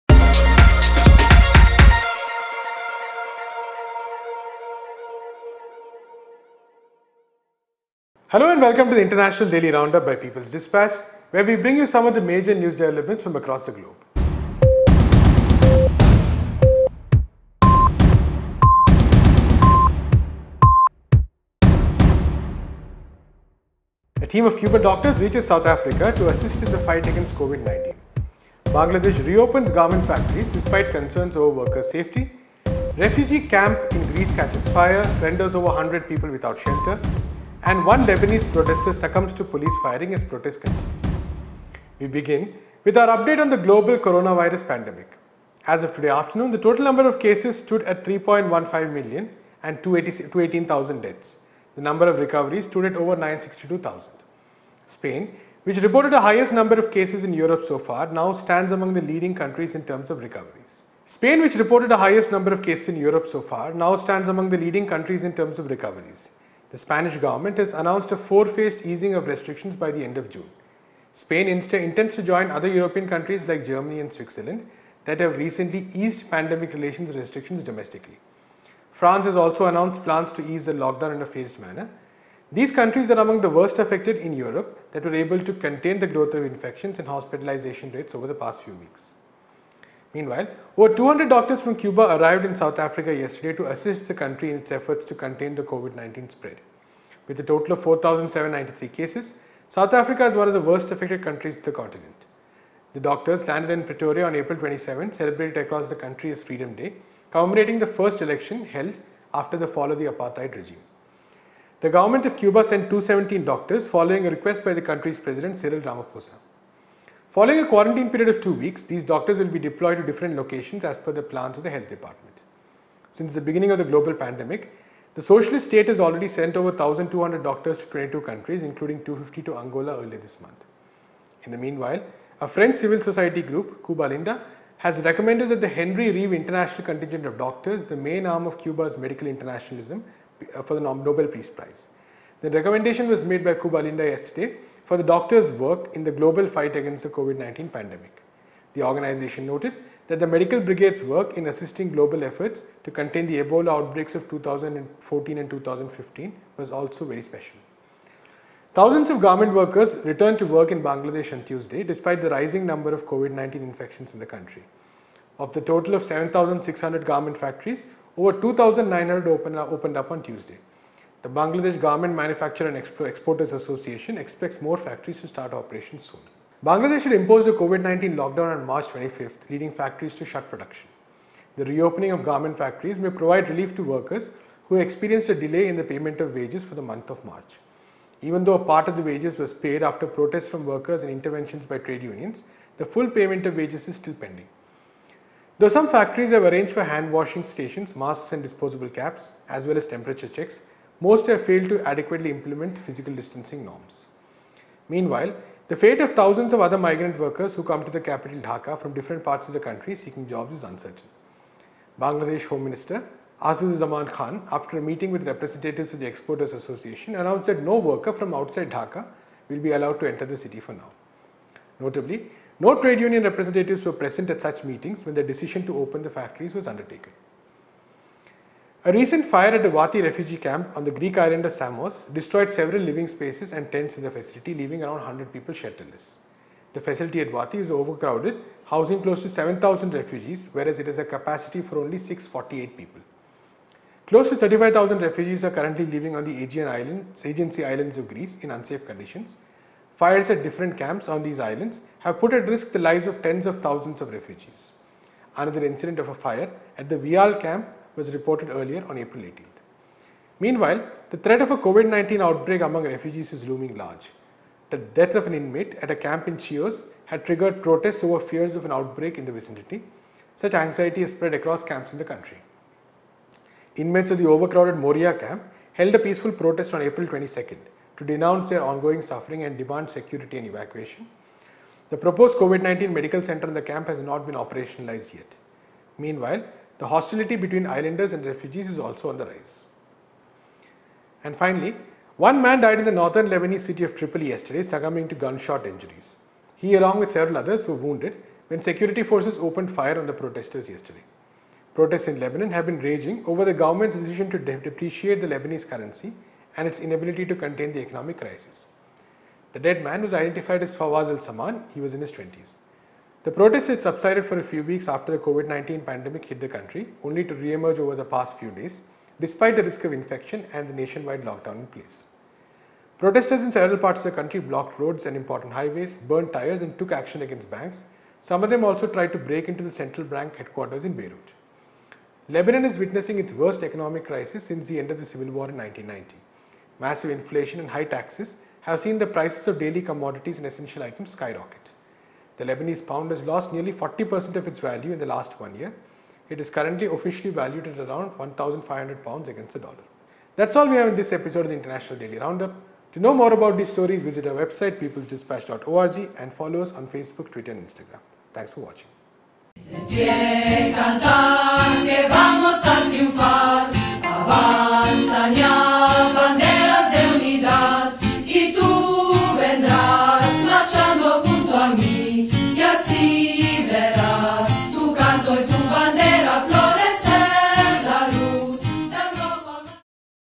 Cuban medical internationalism continues, as doctors reach South Africa
In today’s episode, we take a look at the expansion of Cuba’s medical internationalism as it sends a team of over 217 doctors to South Africa and a campaign for Nobel peace prize for the doctors’ brigade, and more top news of the day.